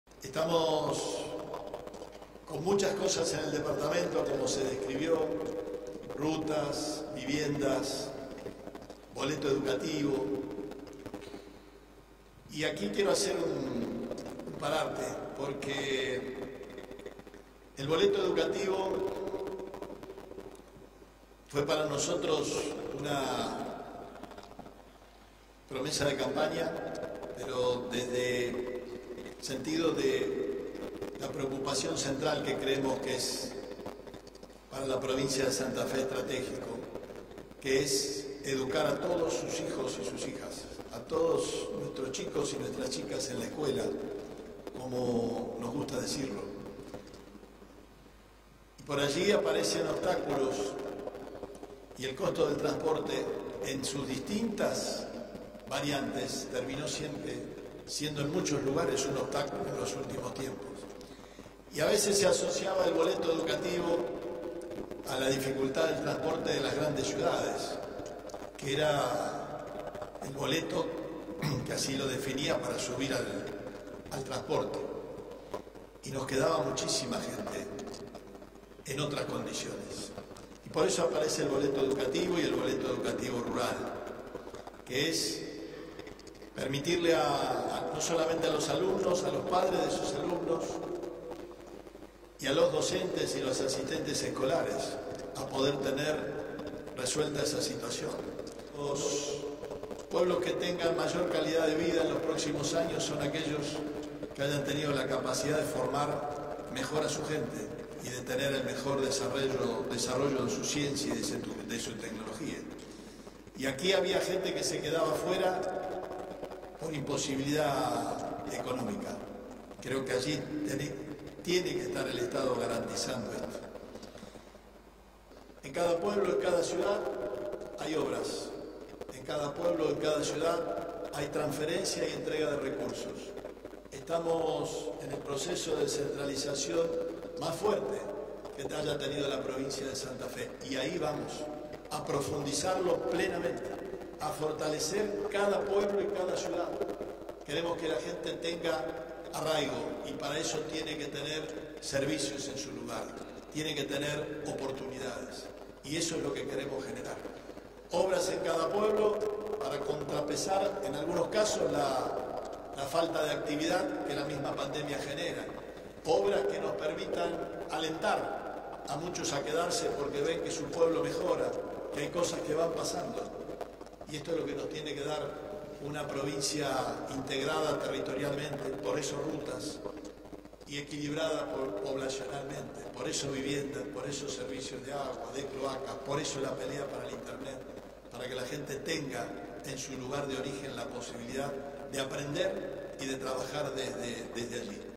PEROTTI ENTREGÓ APORTES POR MÁS DE $3,9 MILLONES A LOCALIDADES DEL DEPARTAMENTO SAN CRISTÓBAL
Corresponden a los fondos de Obras Menores, Emergencias Climática y Gastos Corrientes. Además, firmó convenios de adhesión al Boleto Educativo Rural y entregó escrituras y viviendas.